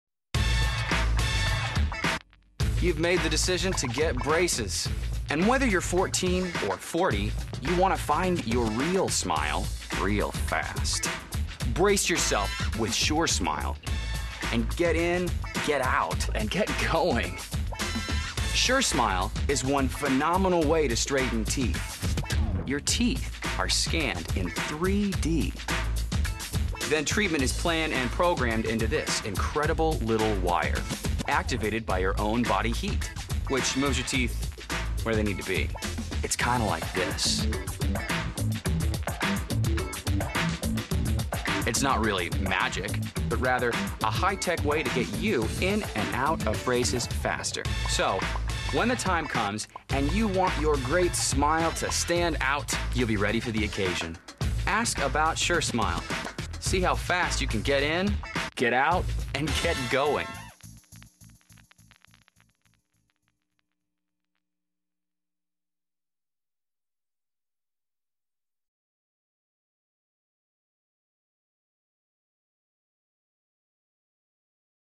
SureSmile - Graber Orthodontics - Orthodontists in Vernon Hills, IL
Vernon Hills orthodontists Drs. Lee & Katie Graber offer SureSmile, a revolutionary new orthodontic technology to get you in and out of braces up to 40% faster!  Using sophisticated 3-D imaging software, your individual wires are custom-shaped by a pair of high-tech robotic arms, resulting in a perfect prescription!